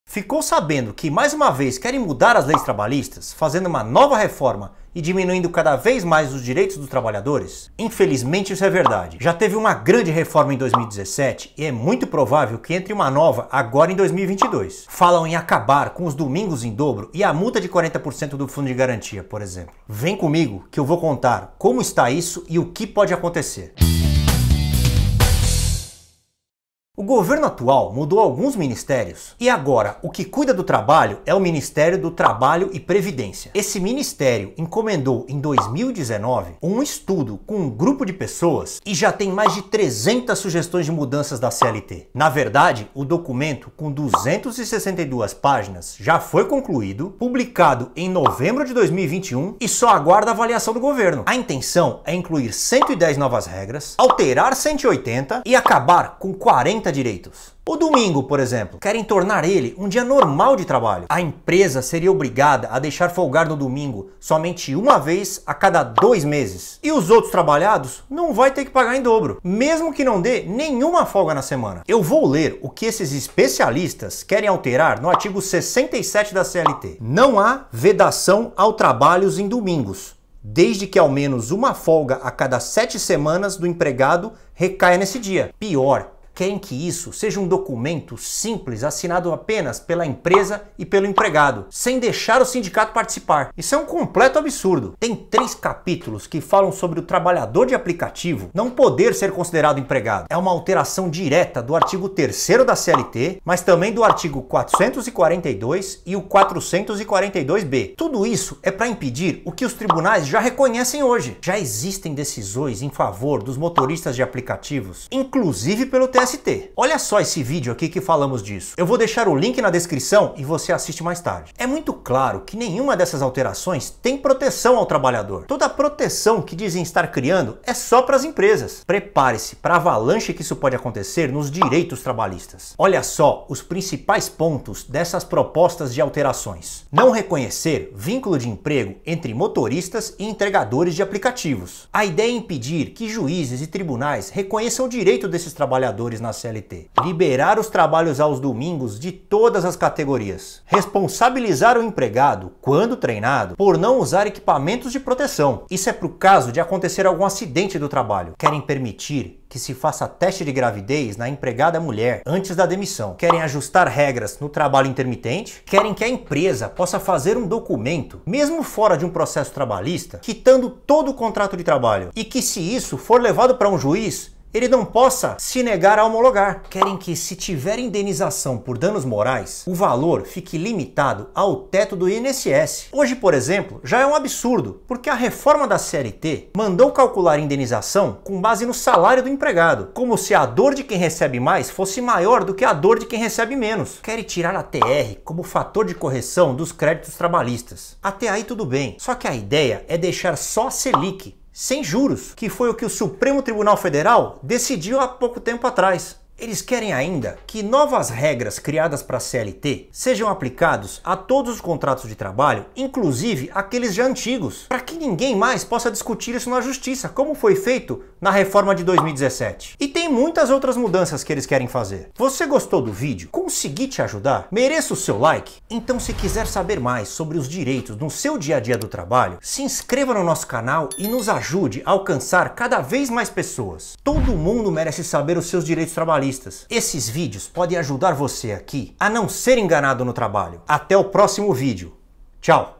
Nova reforma trabalhista em 2022?
Já ficou sabendo que mais uma vez querem mudar as Leis Trabalhistas, fazendo uma nova reforma e diminuindo ainda mais os direitos dos trabalhadores?

Infelizmente é verdade

Já teve uma grande em 2017 e é bem provável que entre outra em 2022

Falam em acabar com Domingos em dobro e multa do FGTS, por exemplo

O Governo atual mudou alguns ministérios e agora o que cuida do trabalho é o Ministério do Trabalho e da Previdência

Esse ministério encomendou, em 2019, um estudo com um grupo de pessoas e já tem mais de 300 sugestões de mudanças da CLT

Na verdade, o documento, com 262 páginas, foi já foi concluído, publicado em novembro/2021, e está sob avaliação do Governo

A intenção é incluir 110 novas regras, alterar 180 e acabar com 40 direitos

O Domingo, por exemplo:
- Querem tornar ele um dia normal de trabalho
- A empresa seria obrigada a deixar folgar no Domingo só uma vez a cada 2 meses
- E os outros trabalhados não vai pagar em dobro, mesmo que não dê uma outra folga na semana

Eu vou ler o que esses especialistas querem alterar no artigo 67 da CLT:
"não há vedação ao trabalho em domingos, desde que ao menos uma folga a cada 7 semanas do empregado recaia nesse dia"

Pior!, querem que isso seja apenas um documentos assinado entre empresa e empregado; sem precisar do sindicato

Tem três capítulos que falam sobre o trabalhador de aplicativo não poder ser considerado empregado.

É uma alteração direita do artigo 3º da CLT, além do 442 e o 442-B

PRINCIPAIS PONTOS DAS PROPOSTAS
- Não reconhecer vínculo de emprego entre motoristas e entregadores e aplicativos. Ideia é barrar decisões judiciais que reconheçam o vínculo e os direitos previstos na CLT
- Liberar trabalho aos domingos para todas as categorias
- Responsabilizar o empregado, quando treinado e equipado, pela falta de uso do EPI (equipamento de proteção individual) em caso de acidente de trabalho
- Previsão de teste de gravidez antes da dispensa da trabalhadora mulher.
- Ajustes nas regras do trabalho intermitente
- Quitação de acordo extrajudicial seria completa, e o juiz, proibido de homologá-lo parcialmente
- Indenização por danos morais com o teto dos benefícios do Regime Geral de Previdência Social como parâmetro, em vez do salário do trabalhador, como previa a reforma de 2017
- Aplicação do IPCA-E (índice de inflação medido pelo IBGE) em vez da TR, como previa a reforma de 2017, ou da Selic em correção monetária de créditos trabalhistas (isso é o que já foi decidido pe STF recentemente e provavelmente o que vão deixar na alteração da CLT)
- Aplicação de leis trabalhistas novas aos contratos já existente, pra ninguém mais discutir na justiça, como foi feito depois da reforma de 2017

Tem alguma dúvida ou quer entrar em contato conosco?